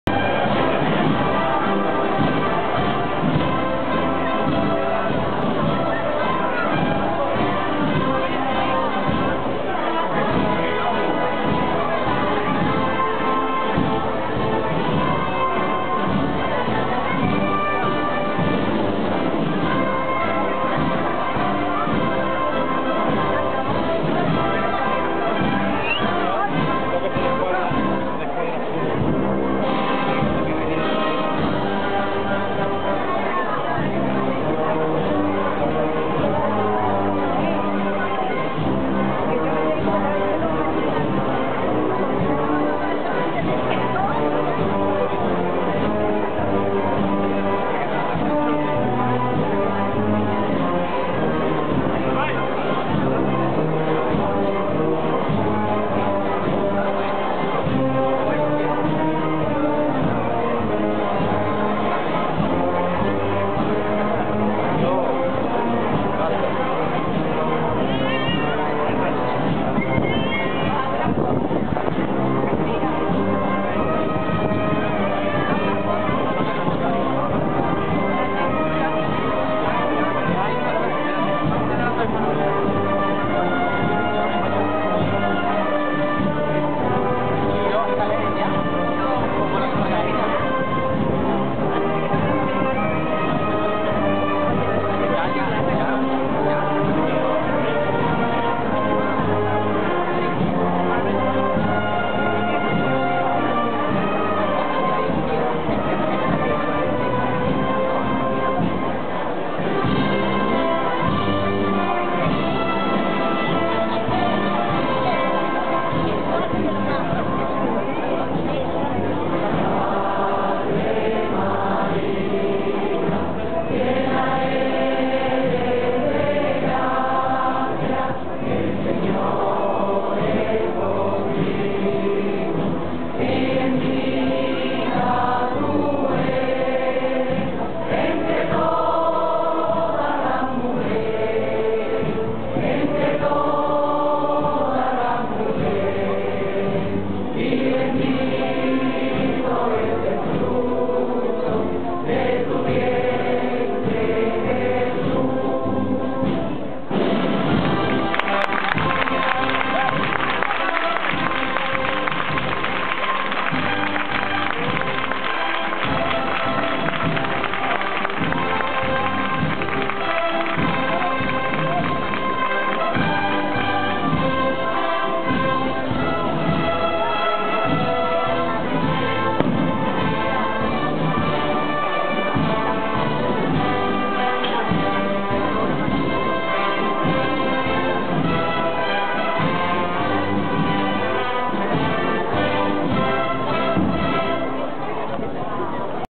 Salida Extraordinaria de la Inmaculada de Castilleja 2011
Salida Extraordinaria de la Inmaculada de Castilleja de la Cuesta, en la tarde del 10 de Septiembre del 2011